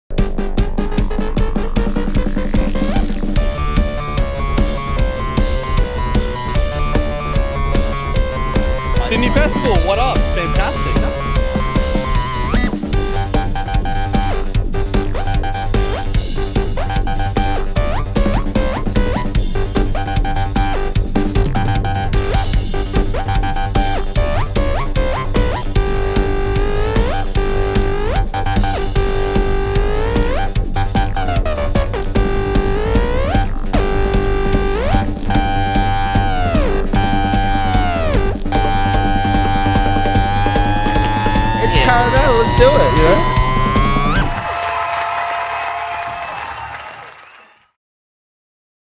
Circus City | Sydney Festival 2018
Sydney Festival is bringing the circus to town and you’re invited!

Circus City returns to Parramatta’s Prince Alfred Square this year, a Festival precinct taking in the Circus Oz Big Top, the Flying Trapeze, Legs On The Wall’s outdoor trampoline playground, and Riverside Theatres.